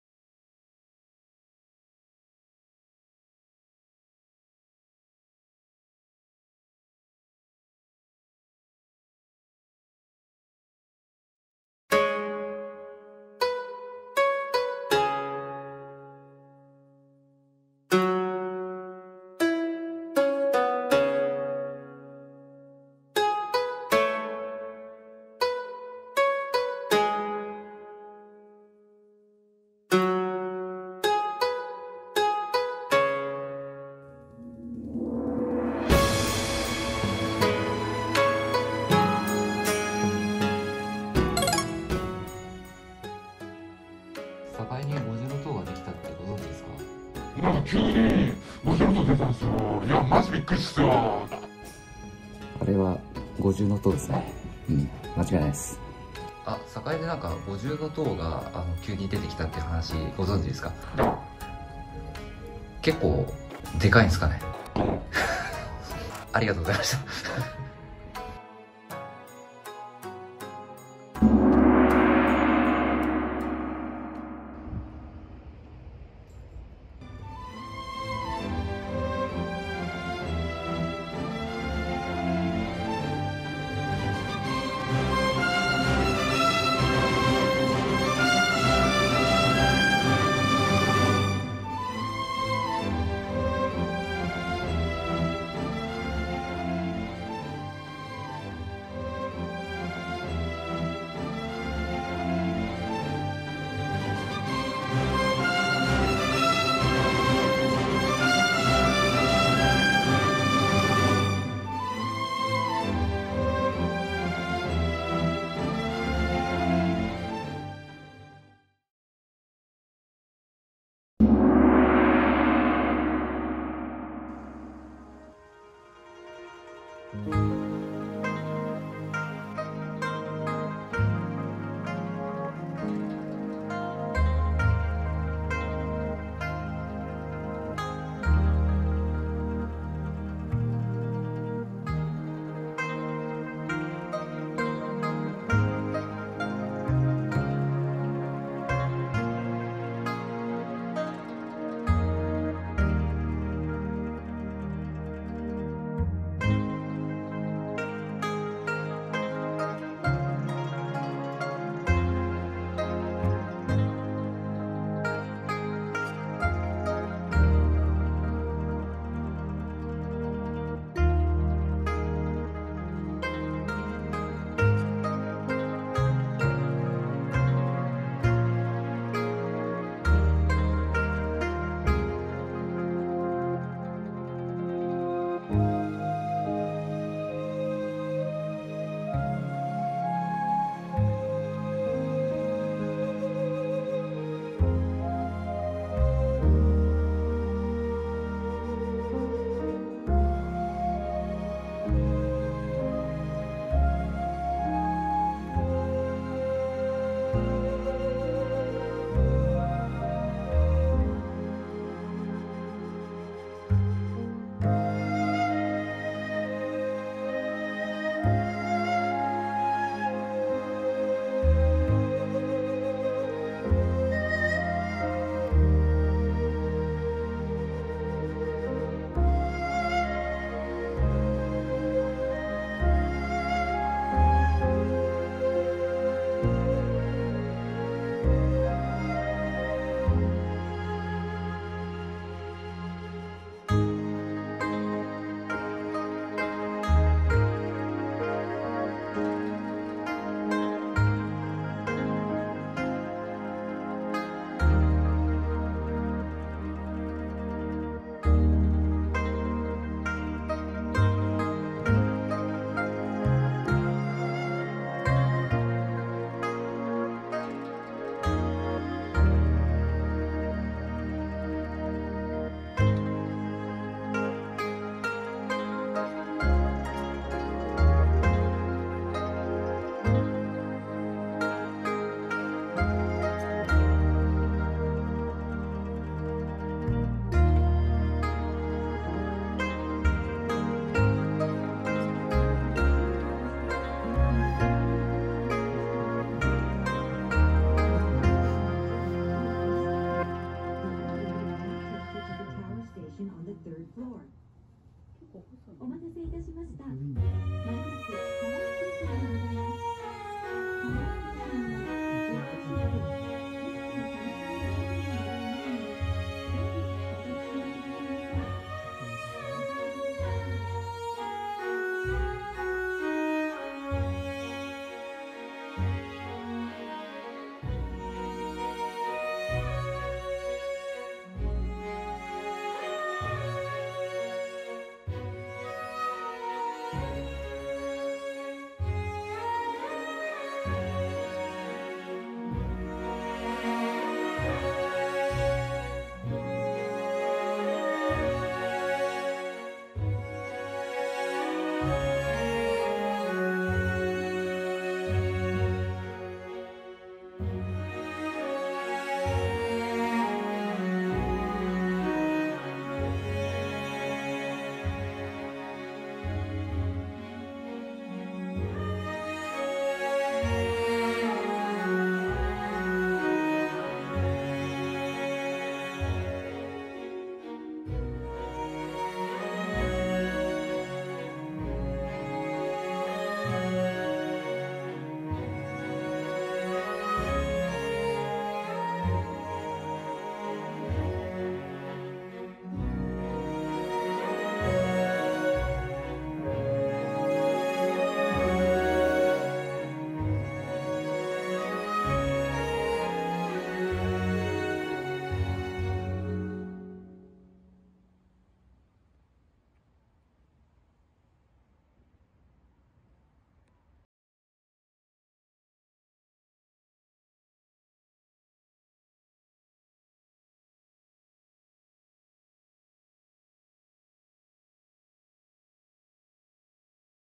名古屋の中心街 栄に突然現れた【五重の塔】その正体とは!? 大人気 久屋大通りパークの夜景も 【RAYARD Hisaya-odori Park】五重塔 今だけ!? 京都? 奈良?
愛知県名古屋市の中心街「栄」に突如「五重の塔」が現れた!？
実際に現地へ足を運び、あちこちから撮影してきました。夕暮れ、夜景といろいろ撮影しましたのでぜひご覧ください。
なかなか威風堂々としてて、立派な塔です。名古屋が京都に!? 奈良に!?

テレビ塔から見た姿も最高です。テレビ塔からは名古屋の街を一望。
五重塔のスケール感も伝わります。

この姿は訳あって12月中旬ごろまで??の姿だとか?
具体的な内容は動画の中に。

今話題の久屋大通りパーク（レイヤード ヒサヤオオドオリパーク）の
テレビ塔からの眺めや、夜景も収めてます。
名古屋の中心街、久屋大通公園の夜景もお楽しみください。

・関連動画
【名古屋】レイヤード久屋大通パークの全貌を一挙ご紹介！【RAYARD Hisaya-odori Park】

●BGM/効果音 オーディオライブラリ 効果音ラボ

Five-storied pagoda